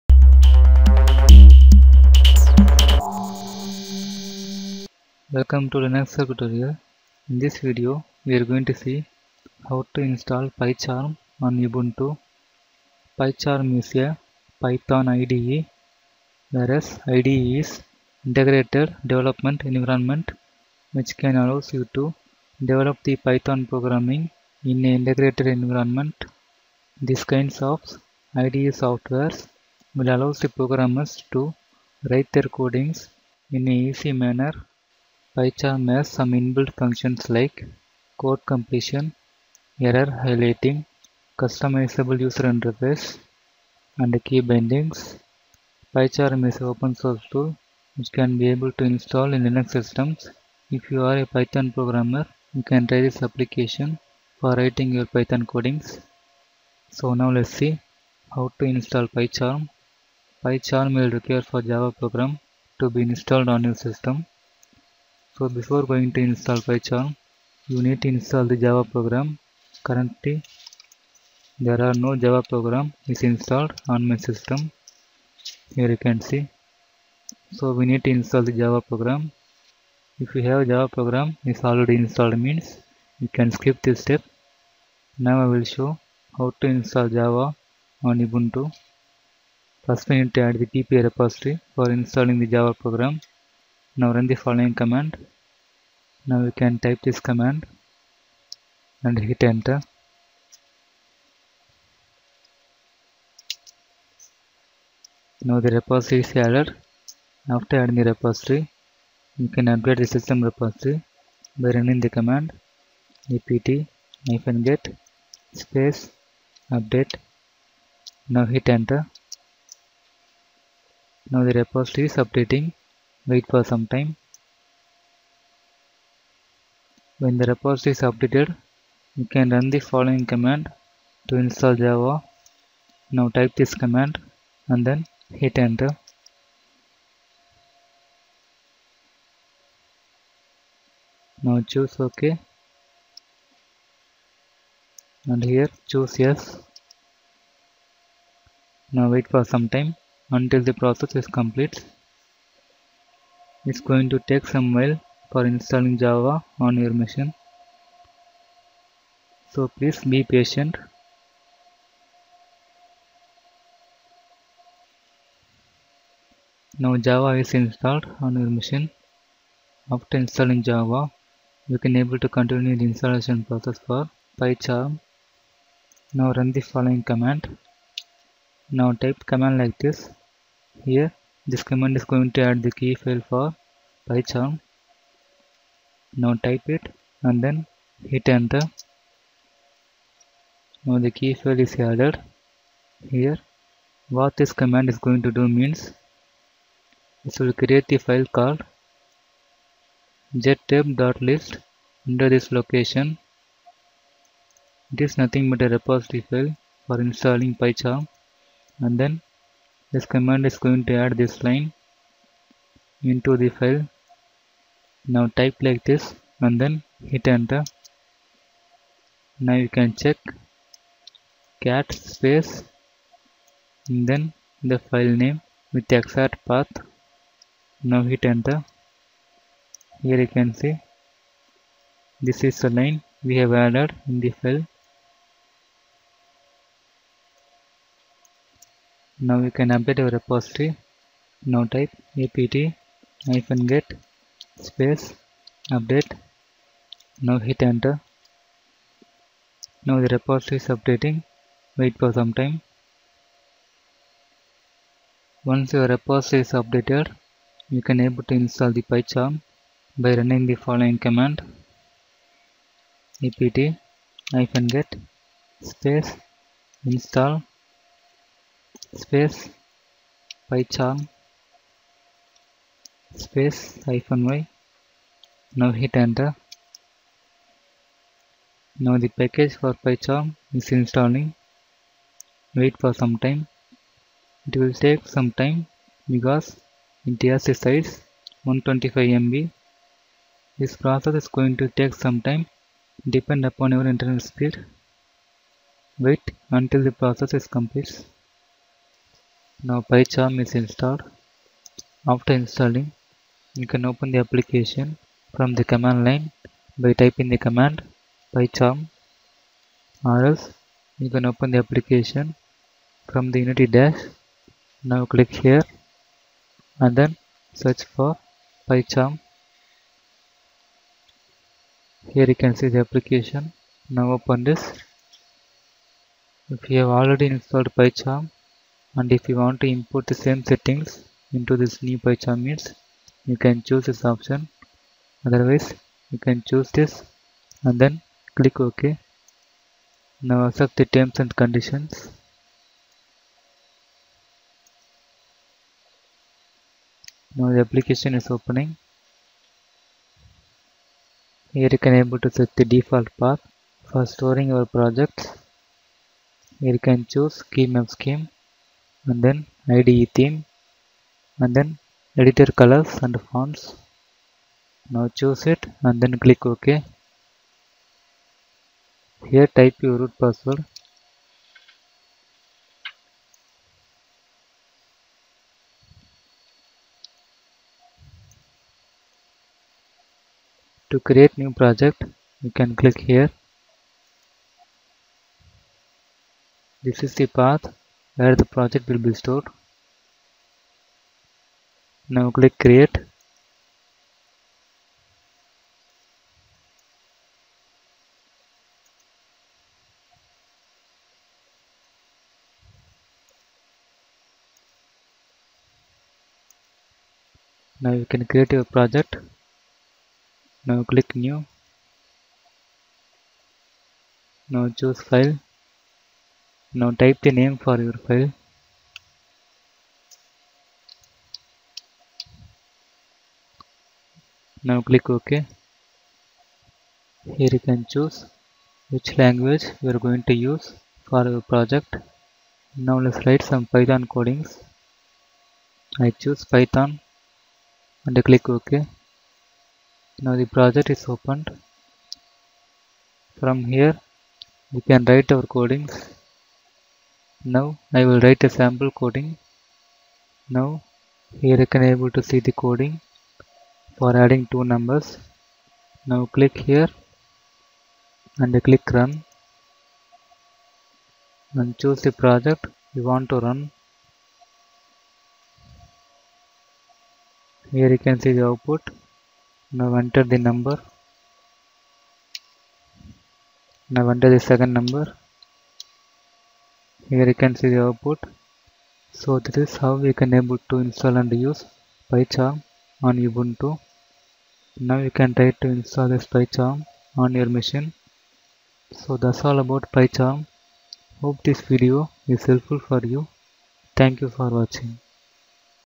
How to install Pycharm in Ubuntu
This video explains the installation of PyCharm in Ubuntu. Pycharm is a free source application to develop python coding.